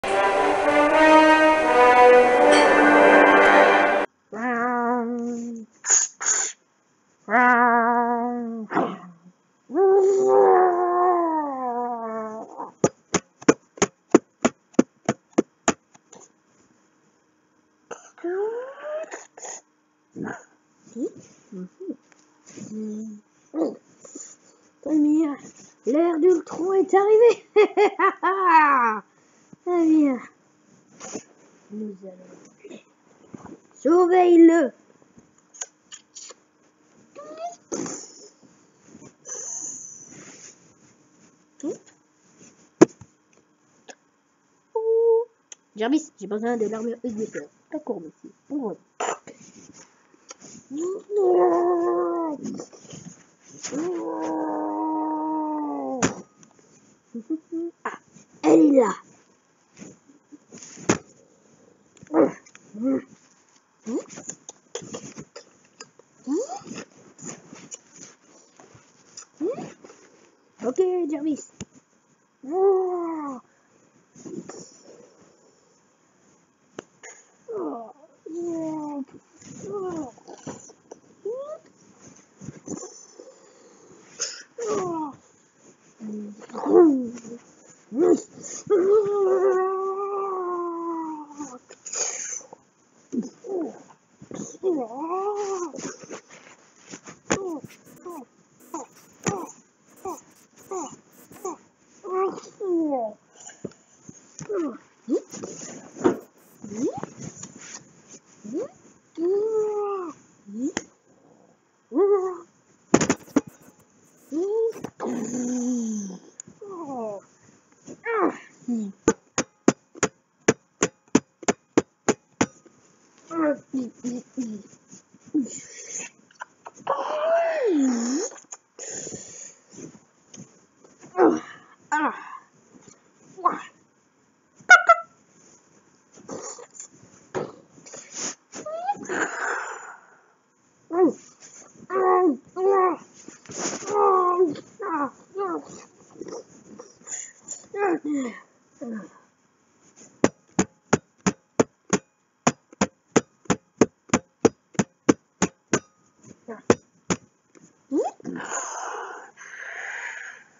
le combat de hulk et iron hulkbusteur - stop motion
Par Hugo - 10 ans